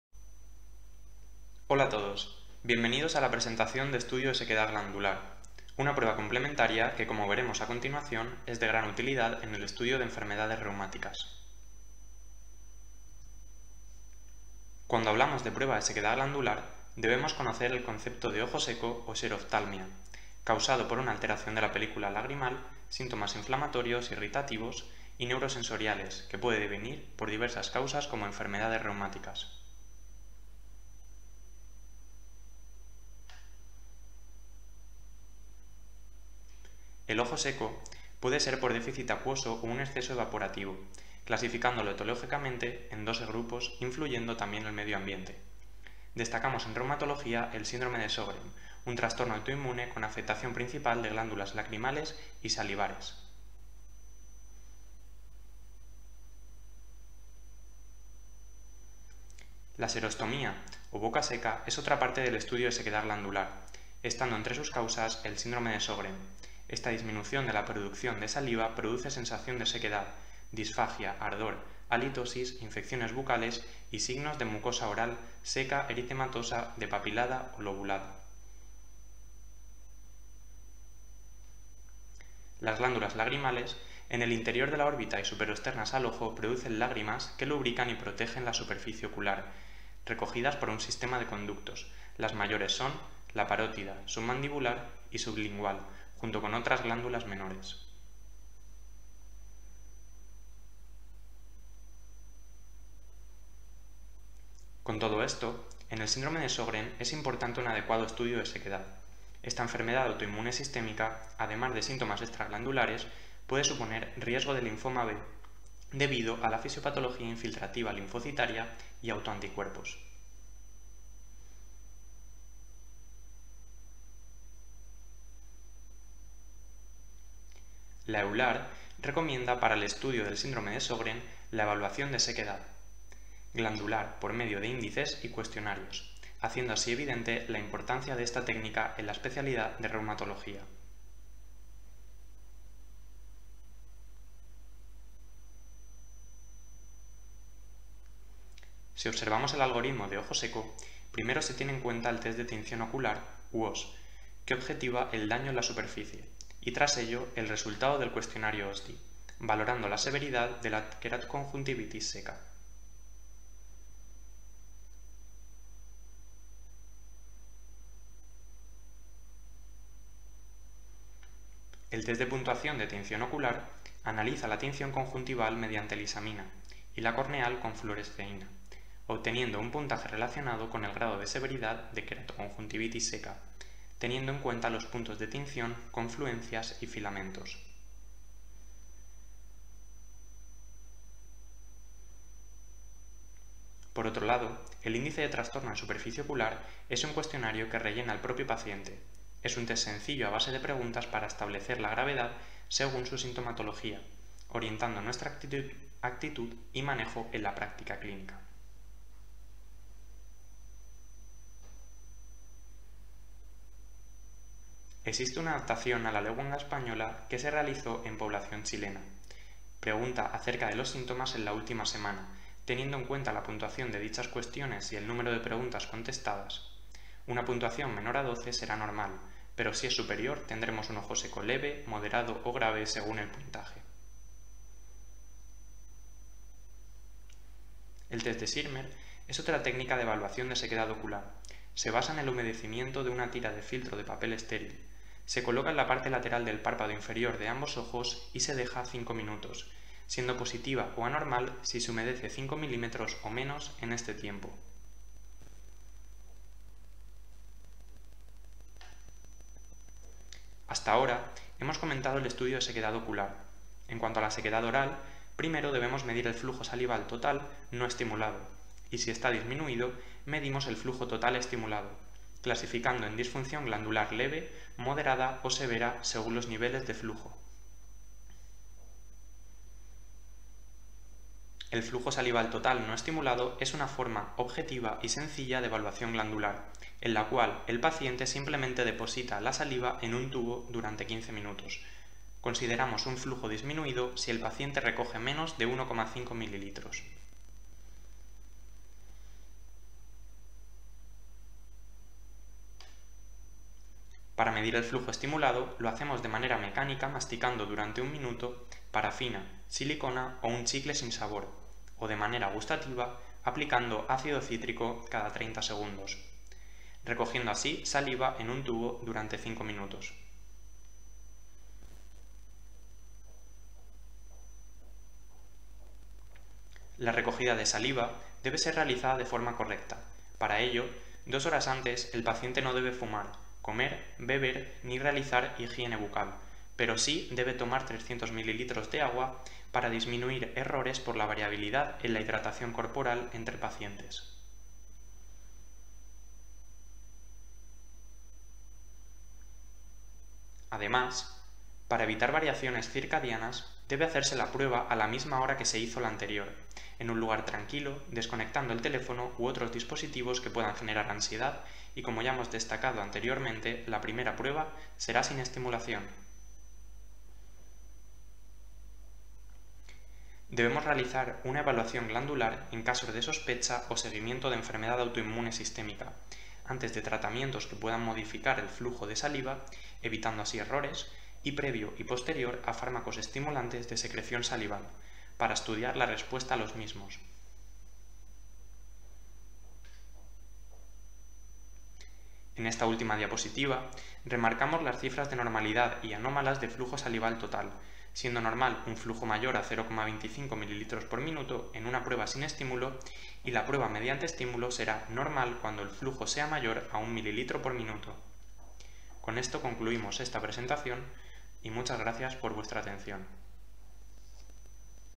Estudio de sequedad glandular en reumatología
Proyecto de innovación docente financiado por el Programa de Innovación Educativa Universitaria PIEU-UMH 2020.
Creado con Microsoft Powerpoint 2019 y grabado mediante OBS Studio v26.1.1.